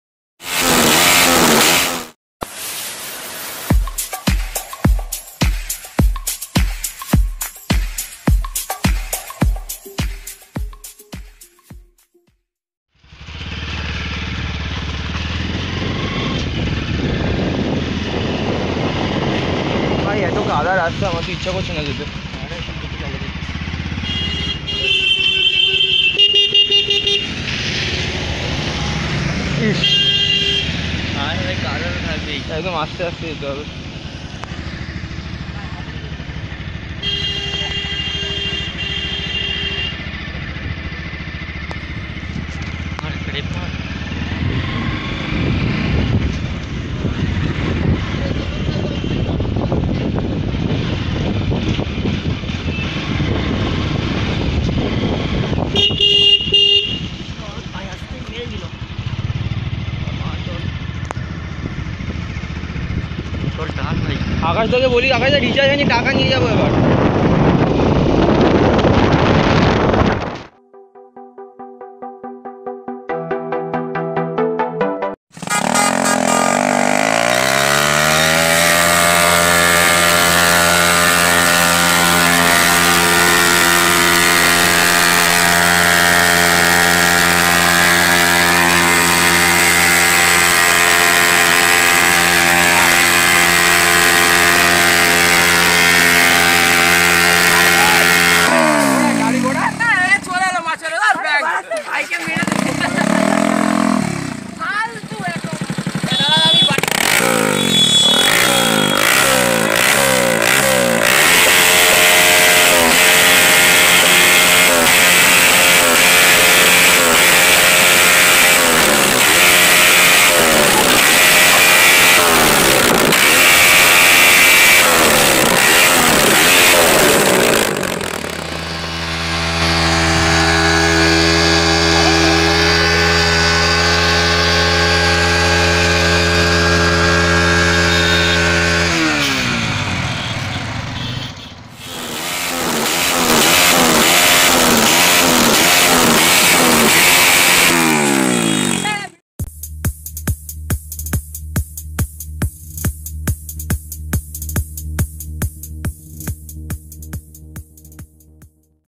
Fire 🔥 Shot From Pulsar 160 Ns | Burn out With Pulsar Ns200 And Ns160 Ready to Drag Race
Best Action camera for YouTube Vlogging

Yoo yoo gyz tj here thanks you so much for supporting lots of love ❤❤❤❤